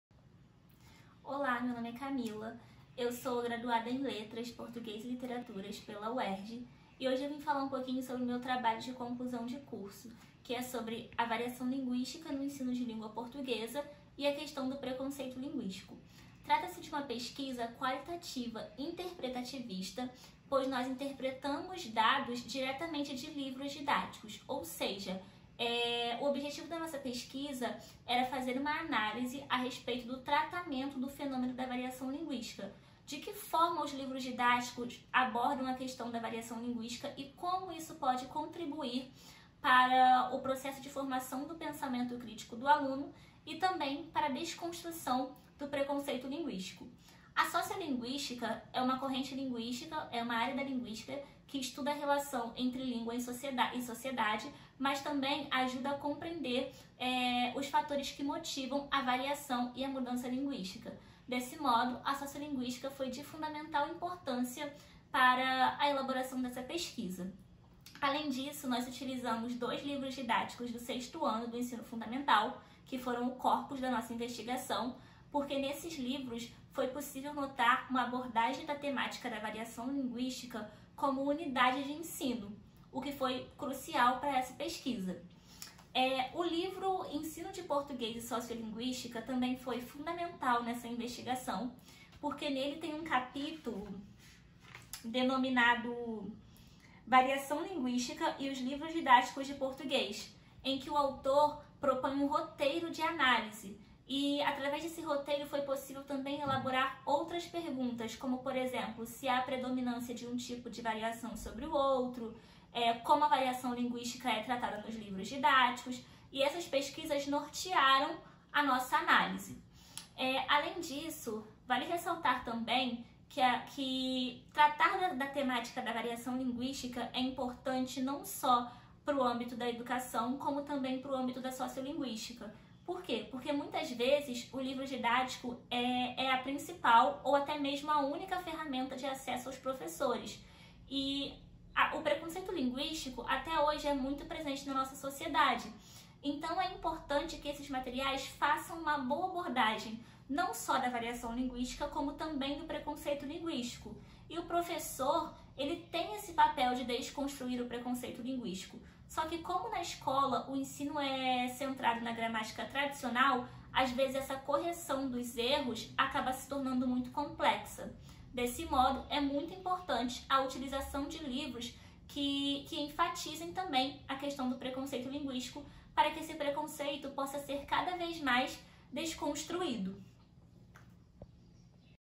Variação linguística no ensino de Língua Portuguesa e a questão do preconceito linguístico.
Monografia apresentada ao curso de letras que tem por objetivo analisar o tratamento do fenômeno  da variação linguística em livros didáticos, buscando encontrar formas criativas, reflexivas e críticas na solução das propostas didáticas que dialoguem com a teoria, desafiando o preconceito linguístico/social que as variantes consideradas estigmatizadas sofrem dentro e fora do espaço escolar. Para isso, realizamos um estudo sobre a sociolinguística, a pedagogia da variação linguística e suas respectivas
contribuições não só no processo de ensino de língua portuguesa como também no processo de formação de pensamento crítico do aluno, visando sempre a desconstrução de paradigmas do preconceito linguístico. Abordamos também as considerações dos documentos oficiais, PCN e BNCC, para o estudo da variação linguística. E, com isso, realizamos uma análise da variação linguística em dois livros didáticos do sexto ano do ensino fundamental, em que a variação linguística é trabalhada como unidade de ensino. Portanto, ao longo desta investigação constatamos que os materiais didáticos que atualmente pertencem ao Programa Nacional do Livro Didático já apresentam alguns avanços na abordagem da temática da variação e são instrumentos cruciais tanto no processo de ensino- aprendizagem quanto no processo de reflexão linguística dos alunos.